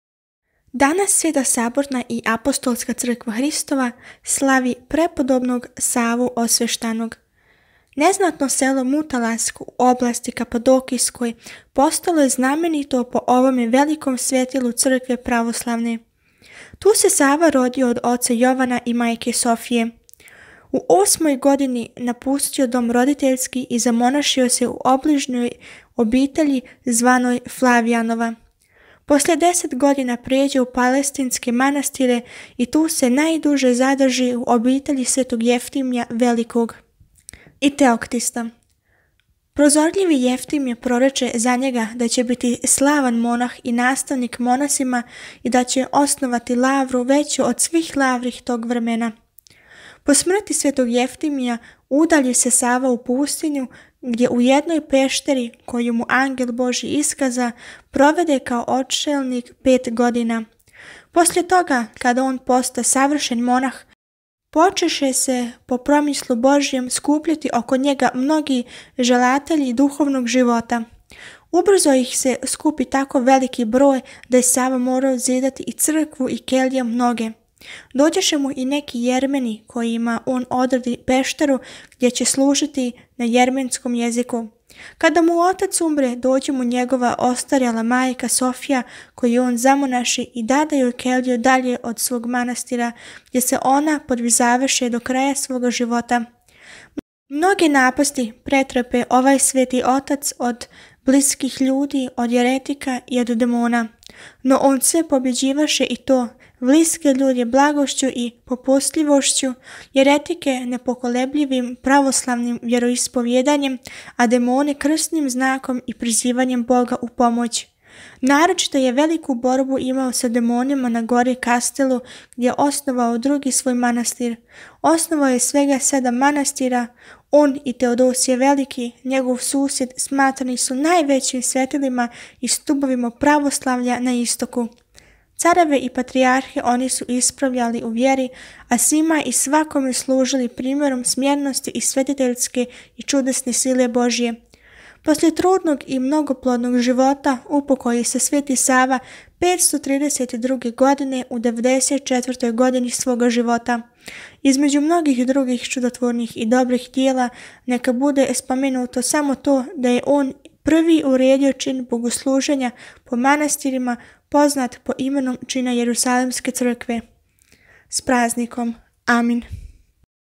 Житије Светих - св. Сава Освећени
Данас се празнује св. Сава Освећени

с Празником

“Охридски Пролог” св. Владике Николаја - страна 802
ТикТок @hadzi_teodora